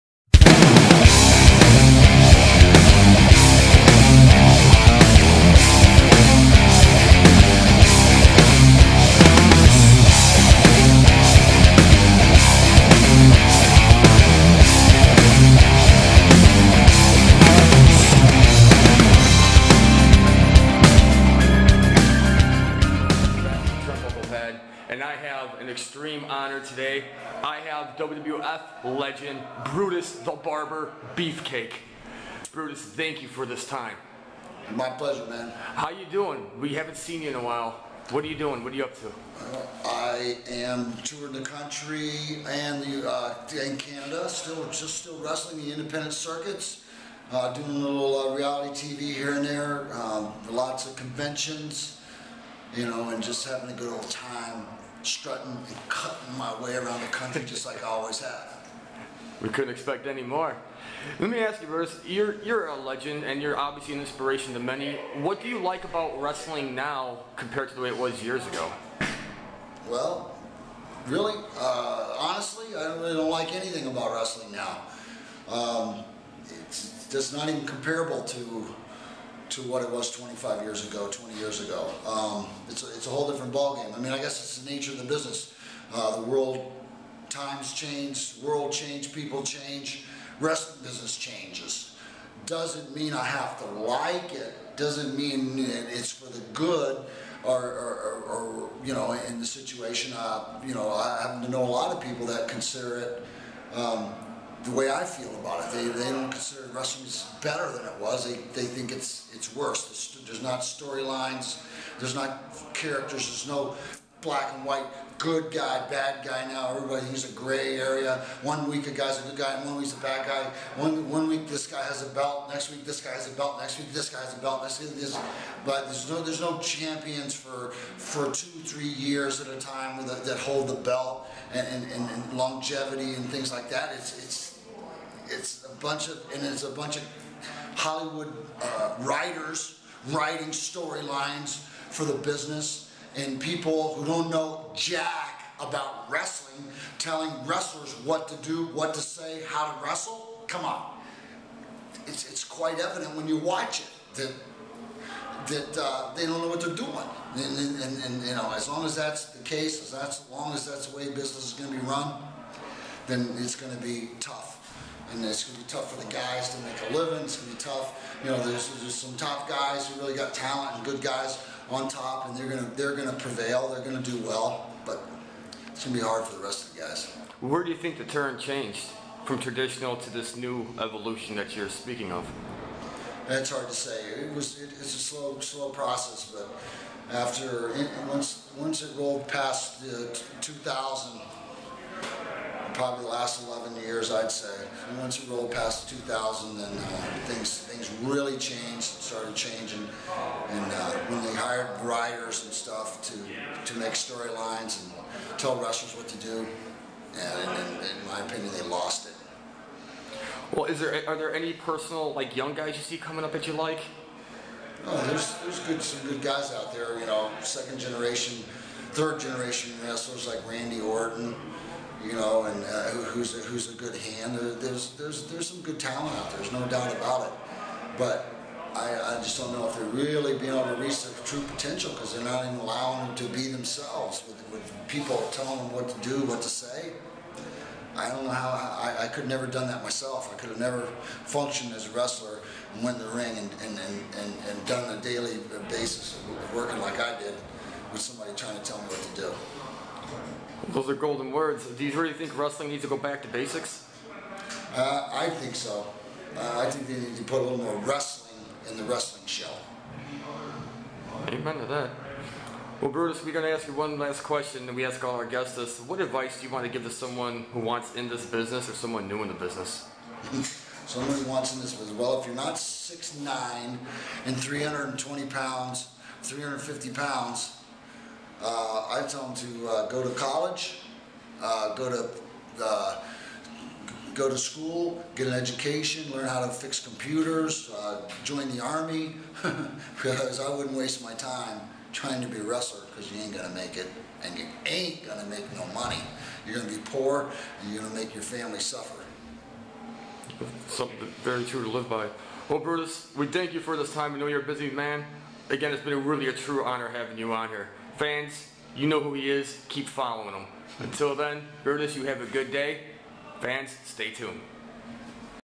TurnbucklePad Brutus The Barber Beefcake
Welcome Back Fans to Our Halloween 2011 TurnbucklePad Special. We have with us one of the greatest old school guys around, He in none other than Brutus The Barber Beefcake. Tune in and check out the what Man behind the blades has to say in this exclusive interview only on The TurnbucklePad!

Powered by Jet Dragon Productions